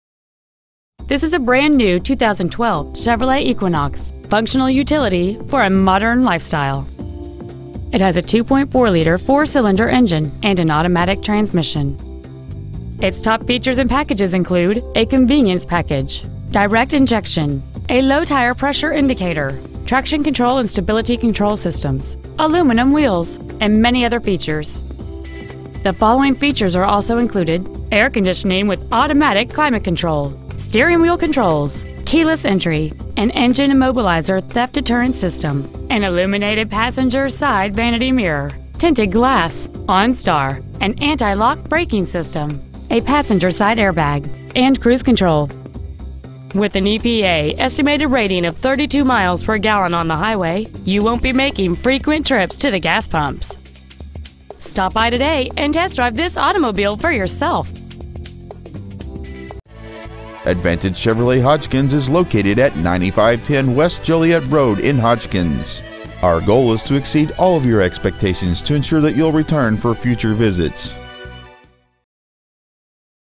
2012 Chevrolet Equinox Hodgkins IL
Year : 2012
 Make : Chevrolet
 Model : Equinox
 Engine : 2.4L DOHC 4-cylinder SIDI (Spark Ignition Direct Injection)
 Trans . : Automatic
 Exterior : Brown
 Miles : 10
 Interior : Black

 2012 Chevrolet Equinox Hodgkins IL